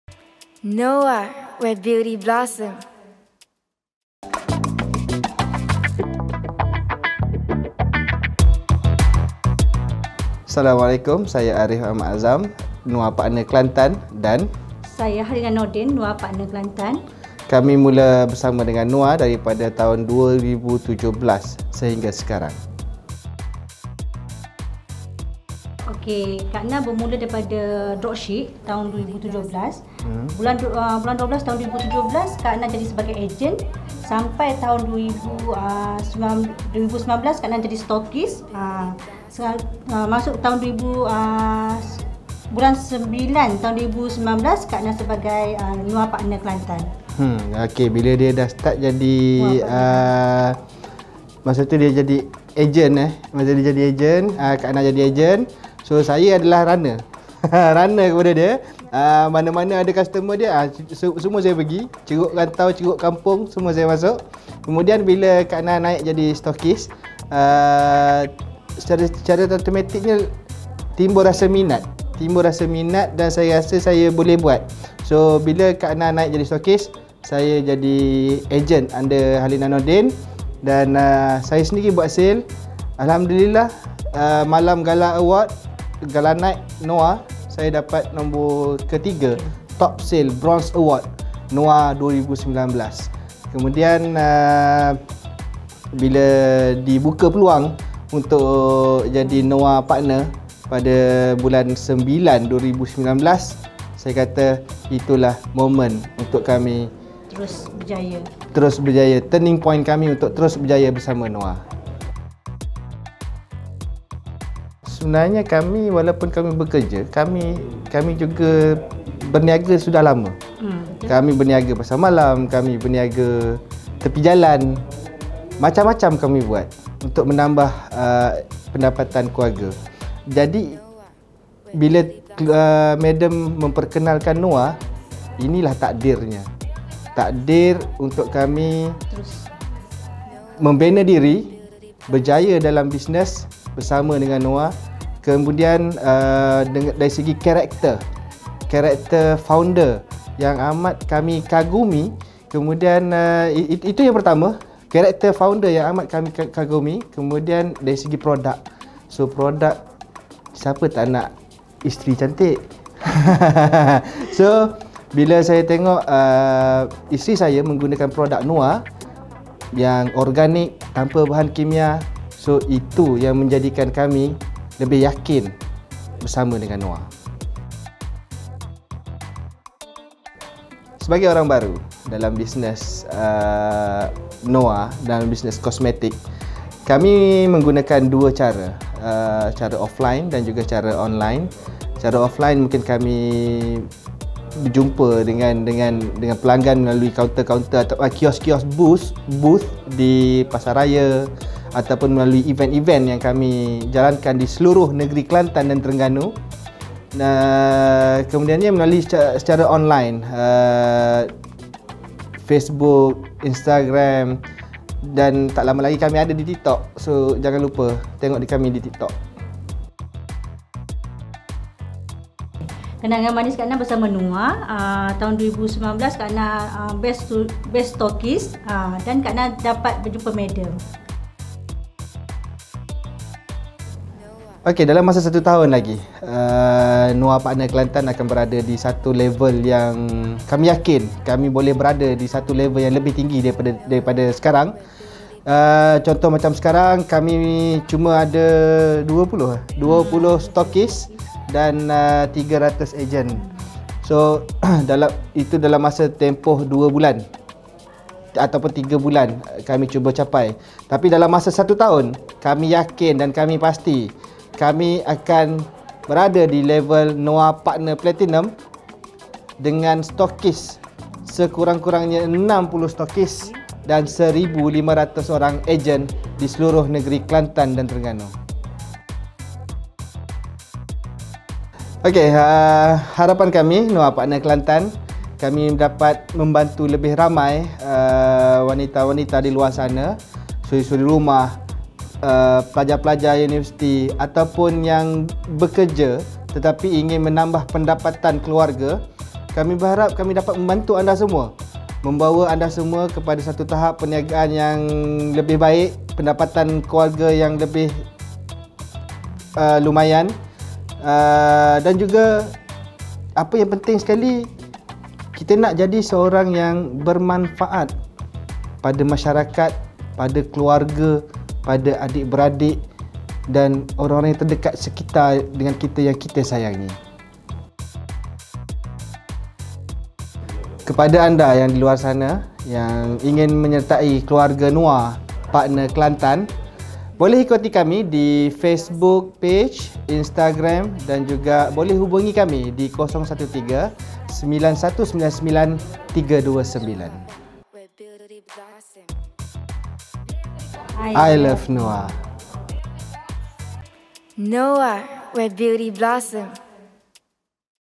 5 MINIT BERSAMA NOIR PARTNER KELANTAN
Dengarkan perkongsian daripada Noir Partner Kelantan tentang bagaimana penglibatan mereka dengan NOIR dapat mengubah kehidupan mereka ke satu tahap yang lebih baik dan berbeza dari sebelumnya.

Jom dapatkan produk Noir hari ini.